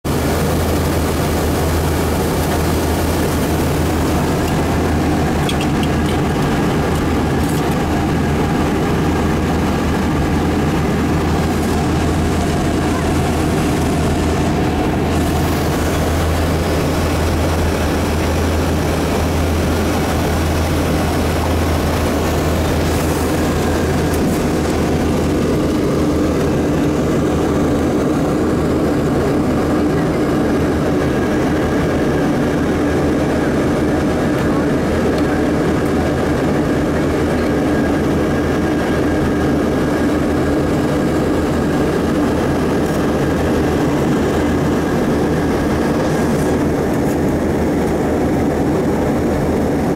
Soundlevel and Cabin view in a YAK-42D /08.2011/FullHD
From Havana to Cancun Mexico
Camera: Sony DSC-HX7V
Cubana